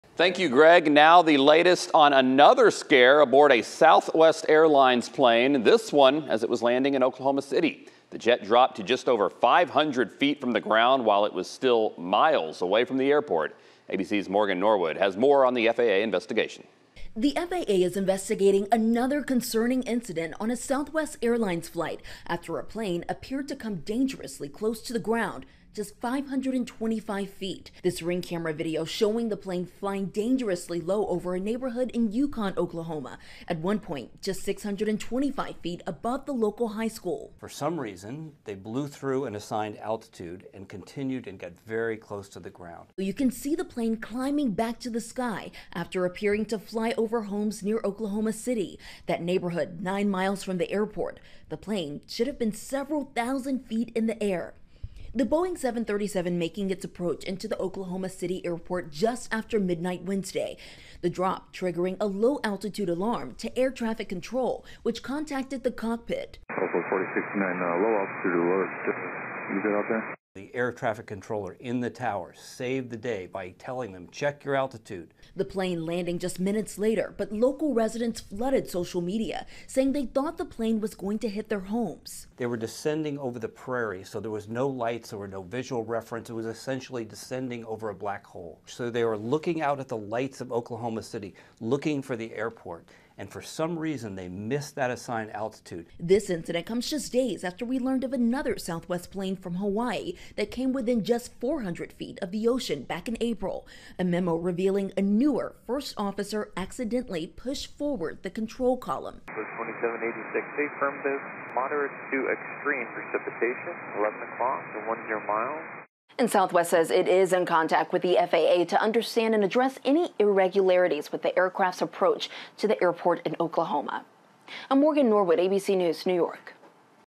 FAA investigating Southwest Airlines low flight over Oklahoma City
The incident happened earlier this week.